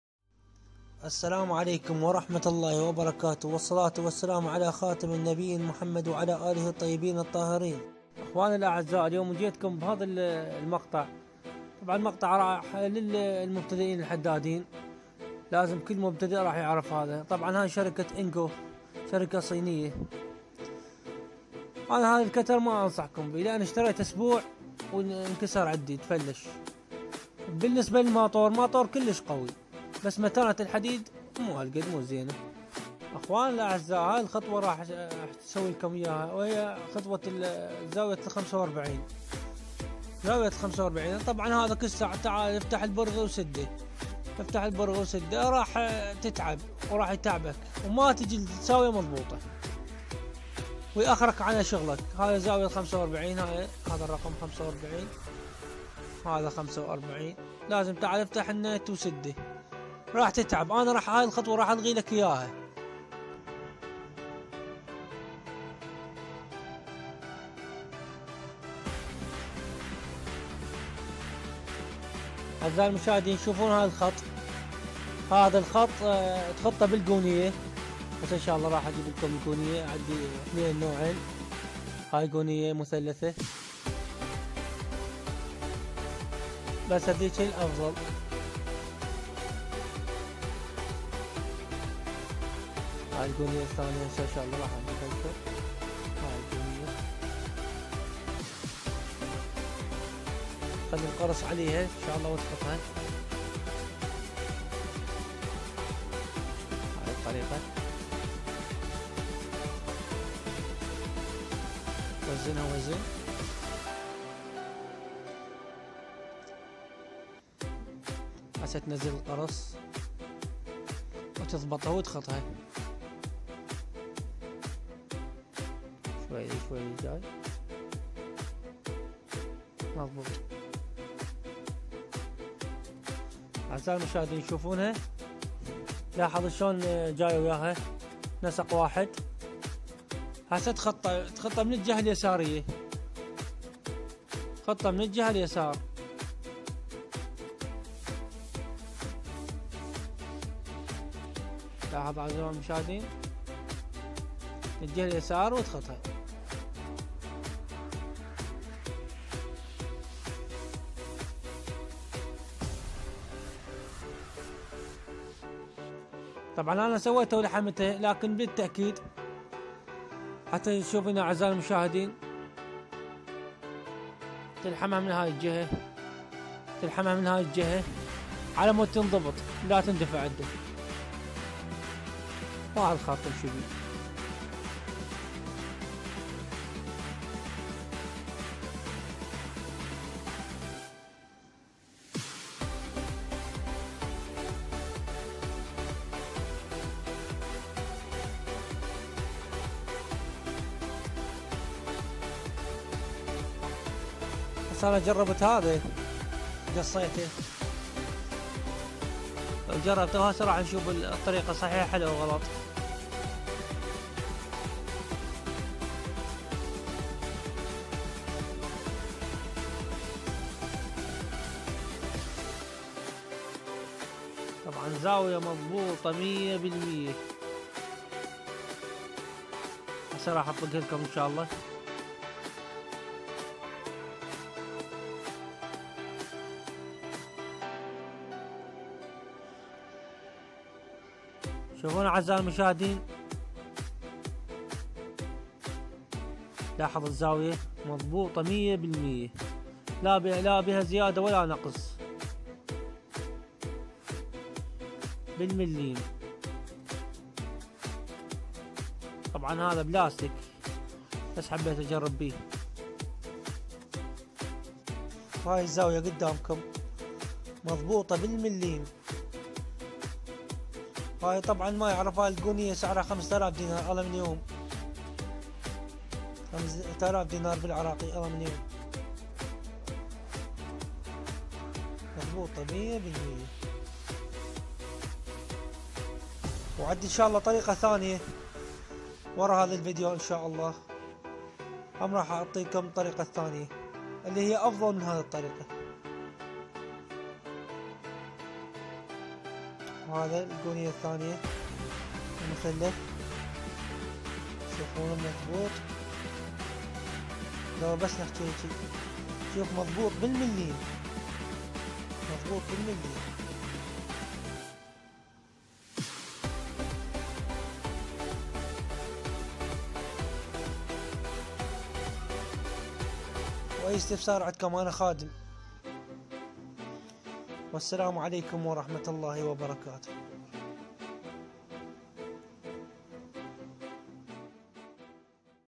قص زاوية قائمة ٤٥ درجة رابط الطريقة الثانية أسفل الفيديو
مرحبا بكم في قناتي المتنوعة علئ اليوتيوب
الطريقة الصحيحة لزاويه ٤٥ درجة ١٠٠٪